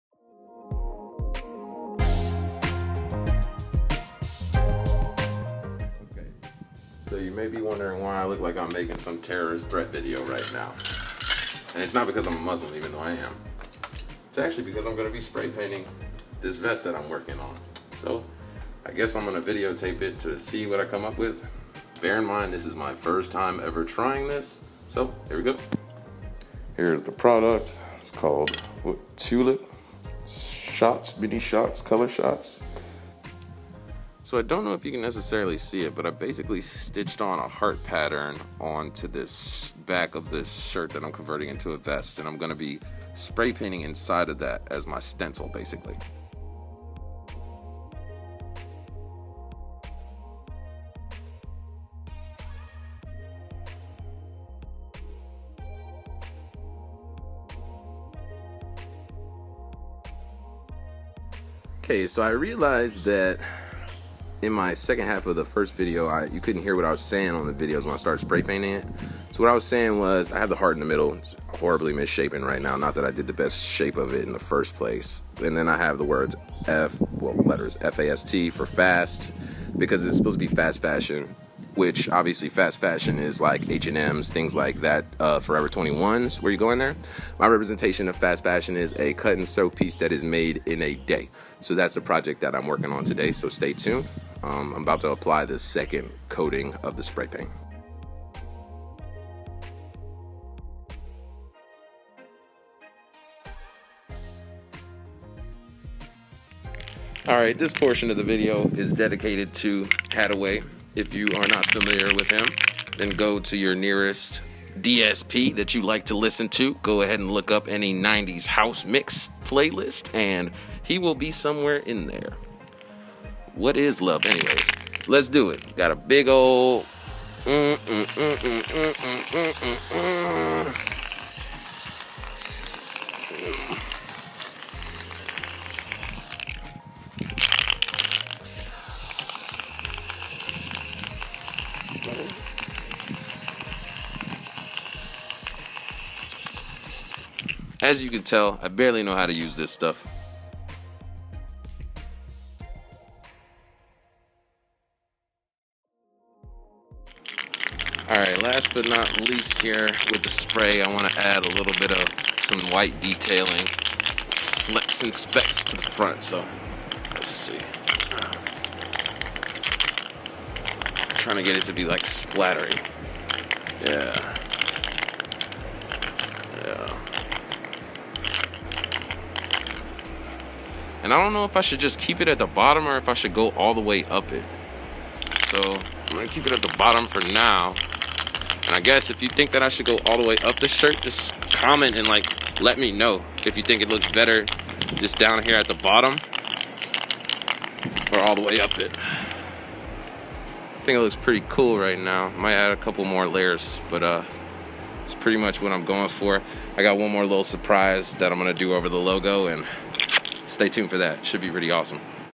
DIY Converting POLO Oxford Into A Vest Pt. 1
I took an old polo by Ralph Lauren oxford and converted into a vest with some fabric spray paint